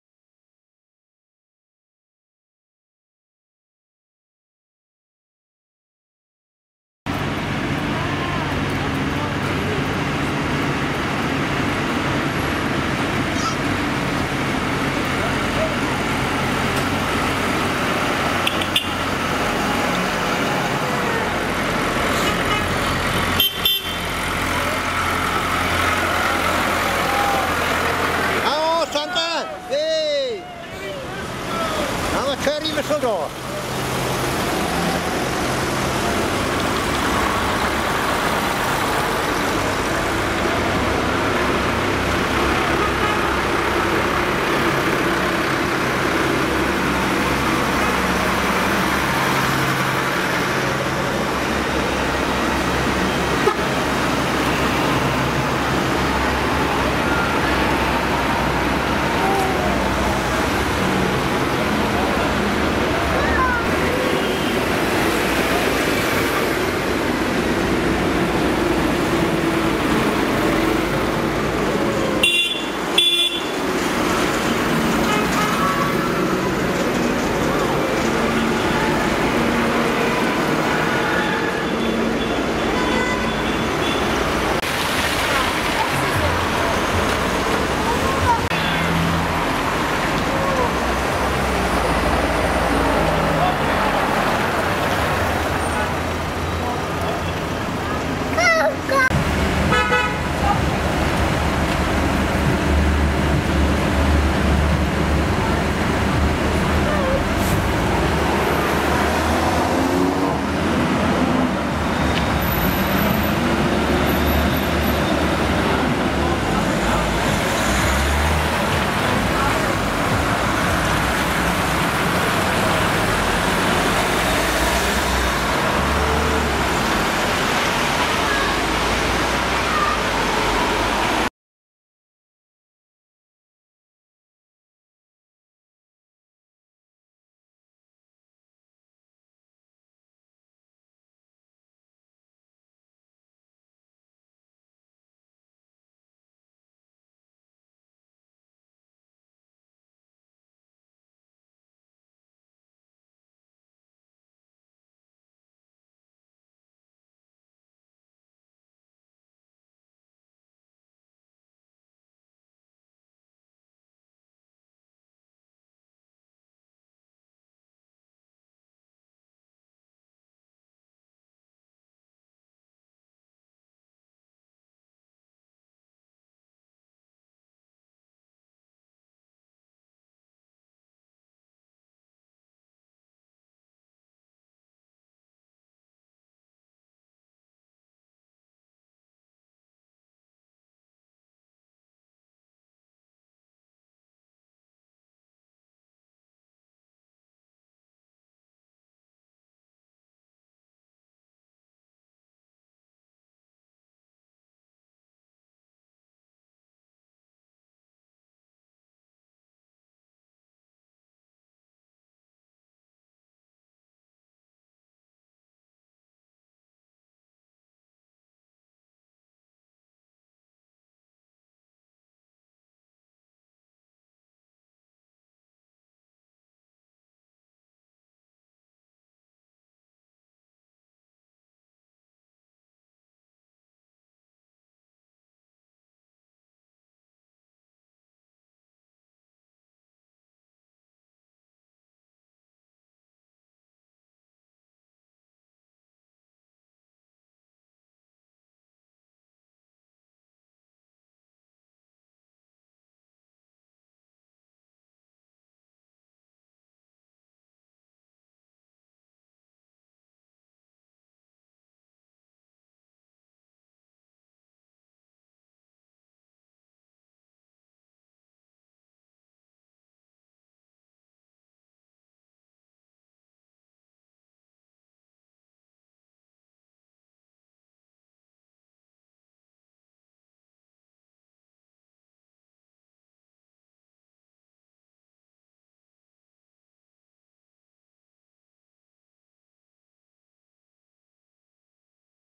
Aberkenfig Santa's tractor run by Glamorgan Ironhorse December 19th 2020
Organised by Glamorgan Ironhorse